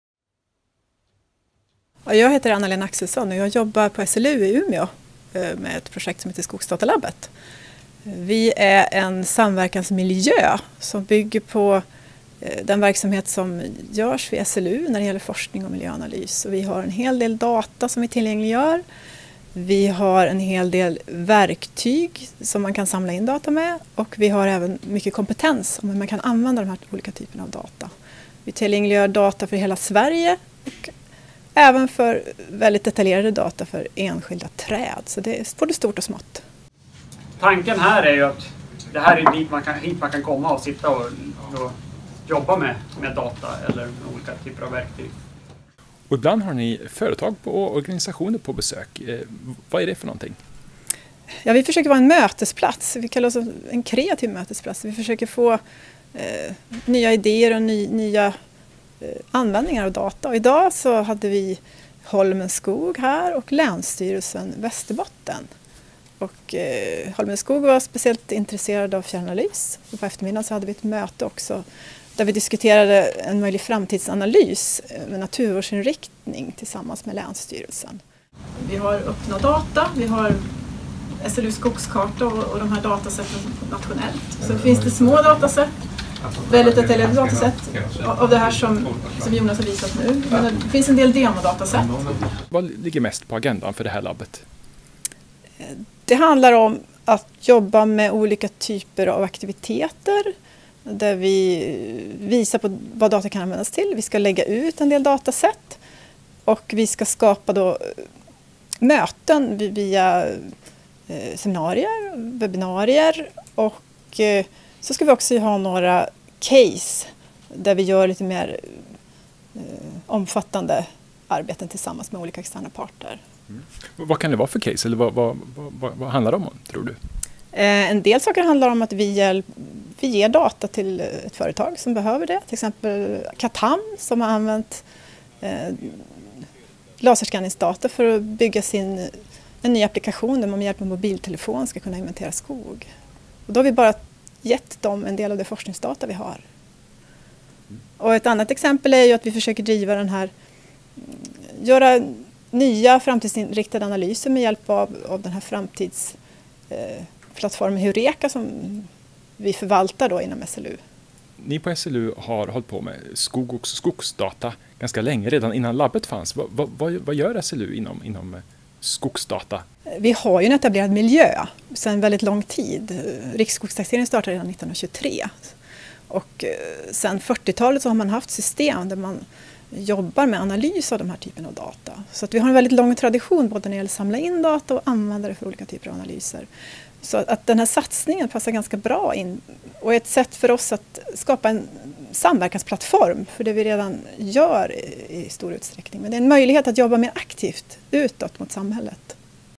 Skogsdatalabbet 2017 - en introduktion
Skogsdatalabbet på Sveriges lantbruksuniversitet i Umeå initierades på hösten 2017. Här är en intervju med Anna-Lena Axelsson, företrädare för Skogsdatalabbet. Intervjun gjordes i december 2017.